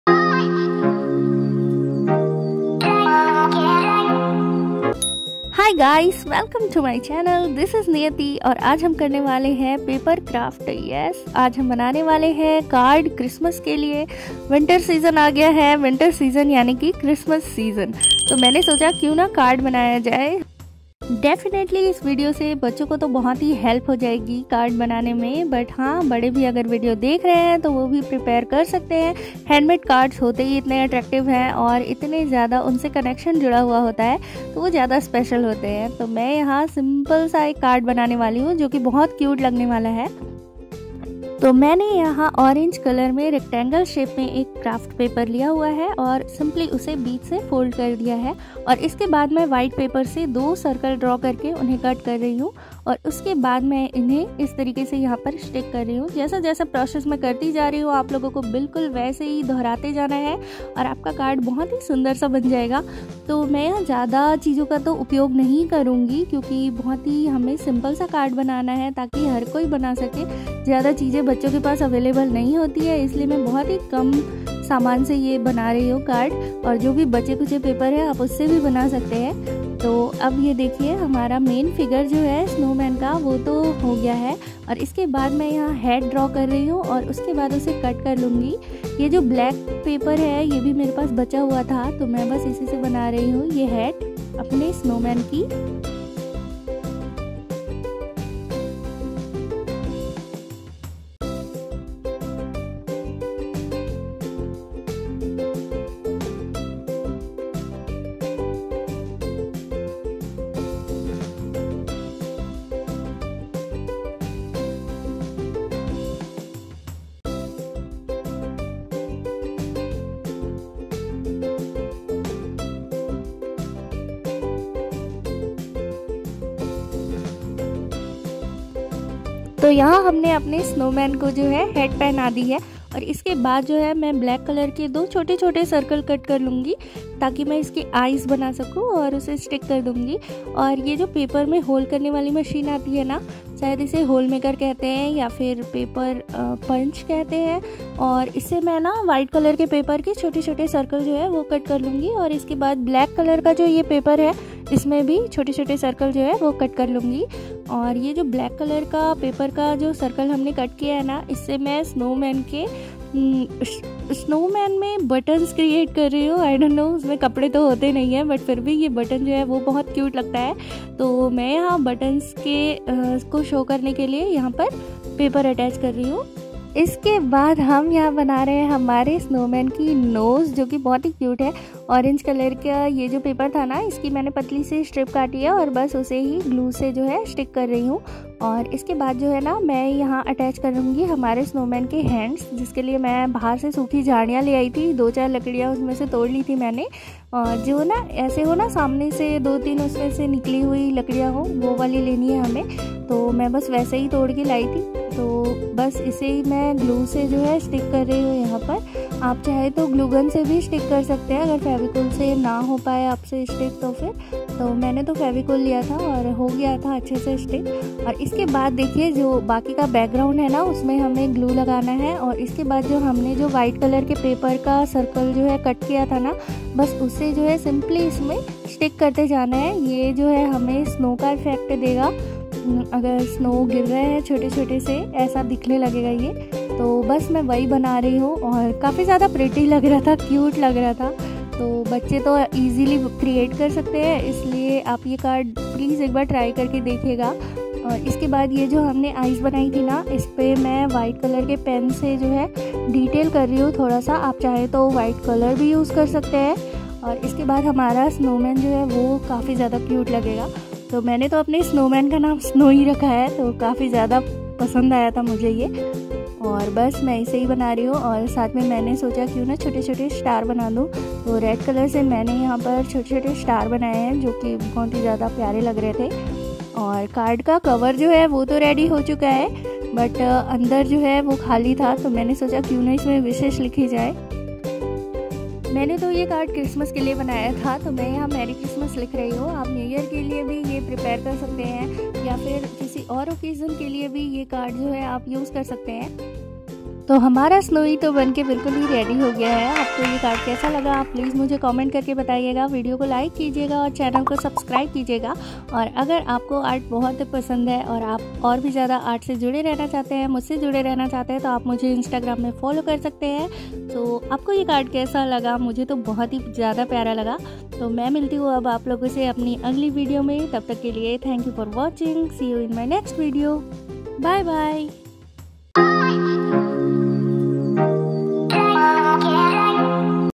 Christmas card idea || How to make paper card || handmade snowman greeting card⛄
Hey,, here i share how to make a Christmas card easily.. You just need some paper and scissors to make this.. I hope you enjoyed watching this and learn something new today.. Thanks for watching,subscribe for more..